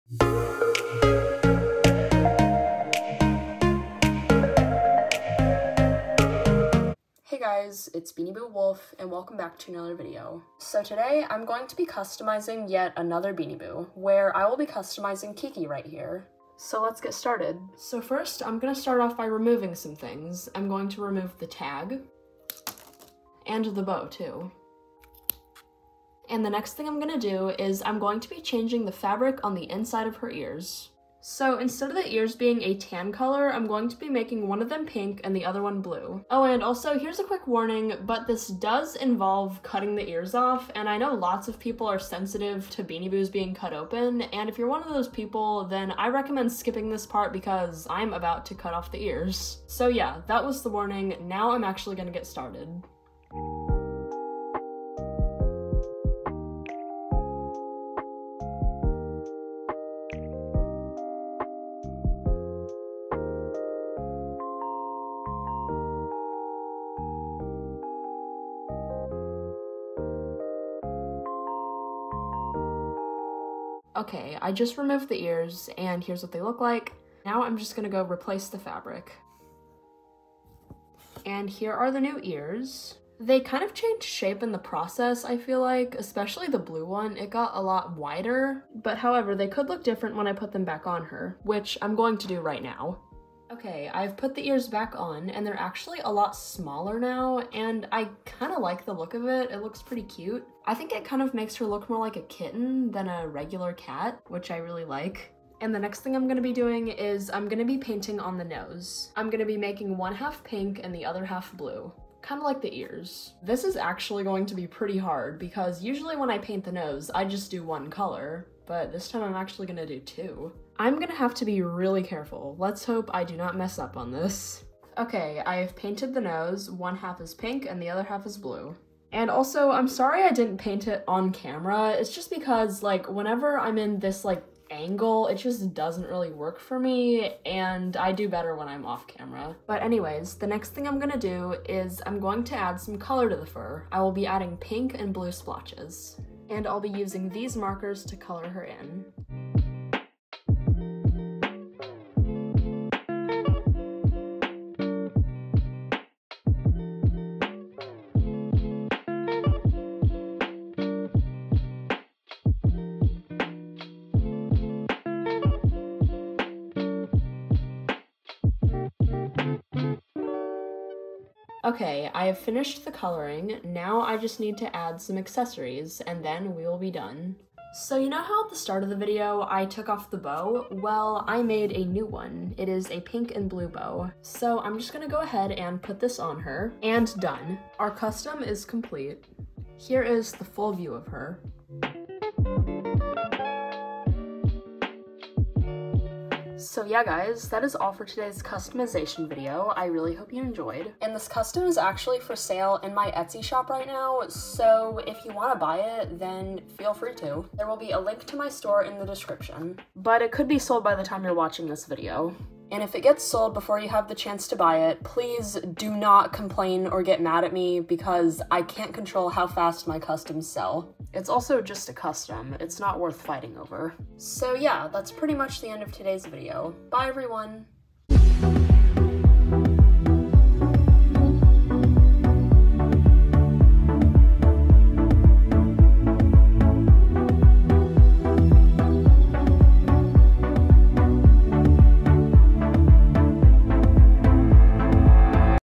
Beanie Boo Custom: Customizing Kiki
Hey guys, welcome to another customization vid! Today I customized Kiki!

My Etsy store: